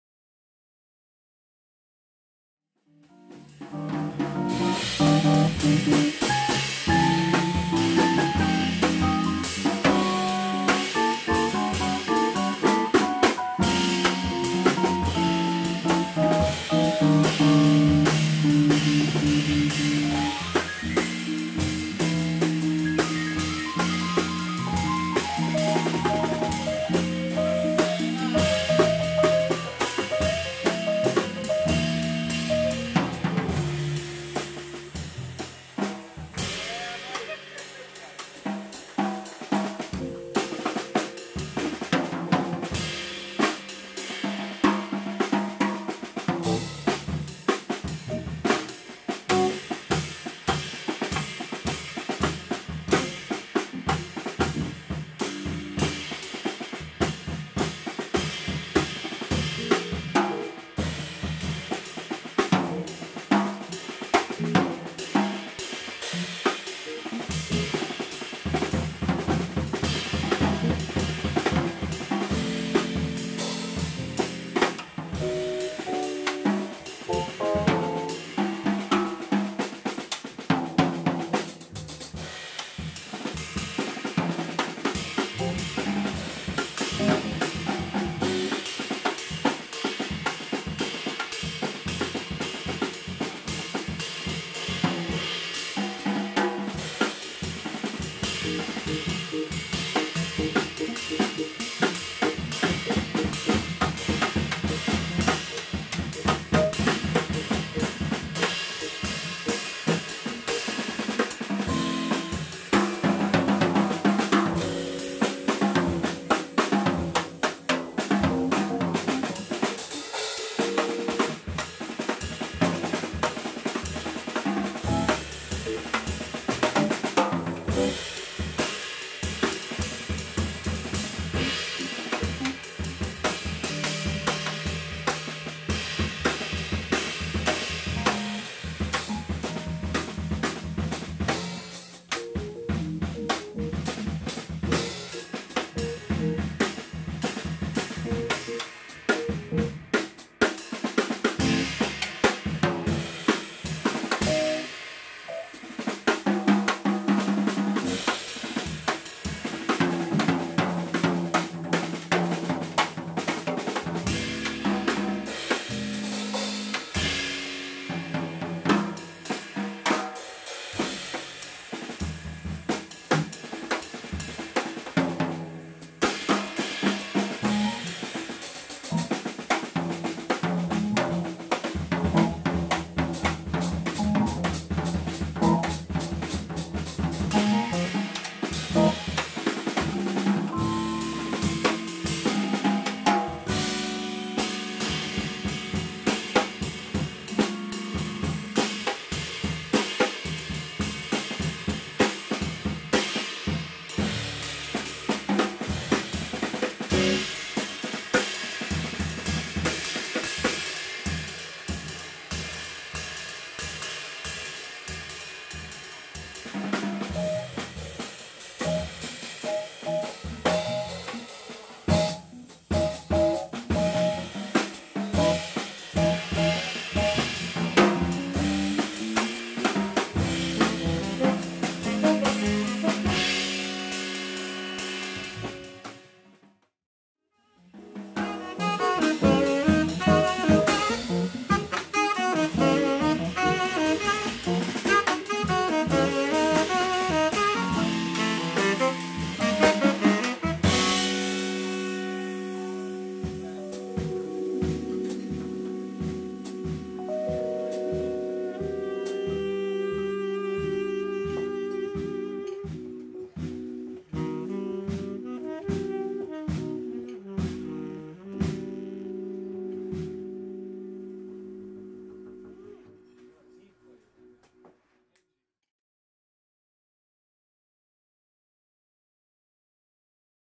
STAN KILLIAN QUARTET - JAZZ (Extract)
At The 55 Bar - 55 Christopher St, New York, NY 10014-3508
Musicians: Stan Killian Sax ; Benito Gonzalez Fender Rhodes ; Corcoran Holt Bass  ; Colin Stranahan Drums
Video, design, realization and production: